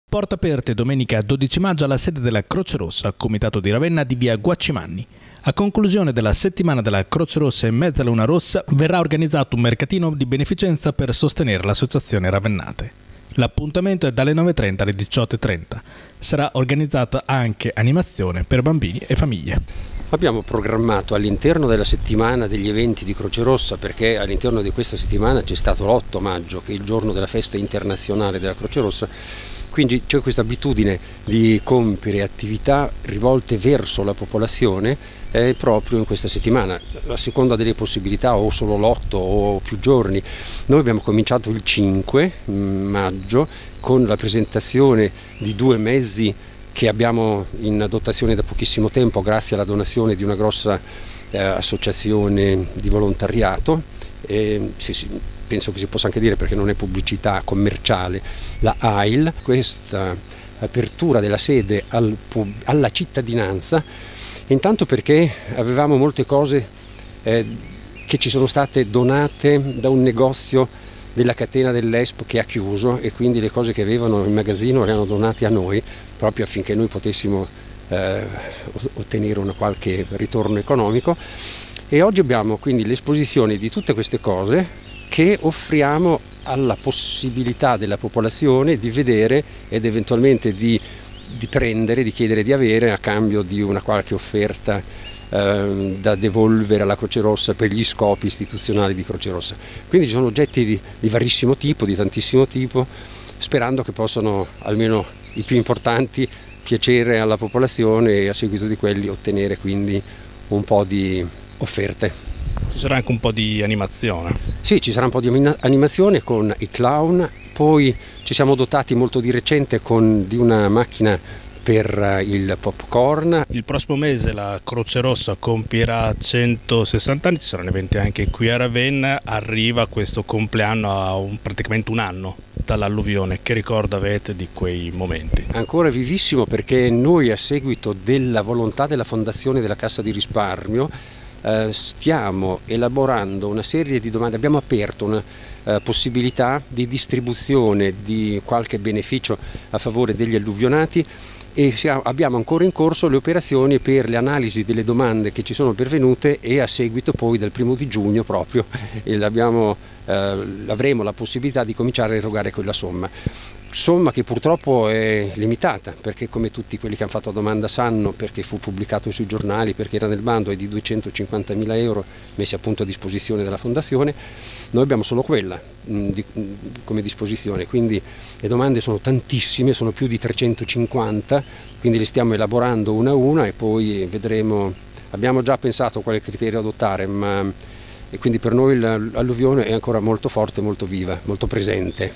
Mercatino per sostenere la Croce Rossa: porte aperte nella sede di via Guaccimanni
Porte aperte domenica 12 maggio alla sede della Croce Rossa, comitato di Ravenna, di via Guaccimanni. A conclusione della settimana della Croce Rossa e Mezzaluna Rossa, verrà organizzato un mercatino di beneficenza per sostenere l’associazione ravennate. L’appuntamento è dalle 9.30 alle 18.30. Sarà organizzata anche animazione per bambini e famiglie.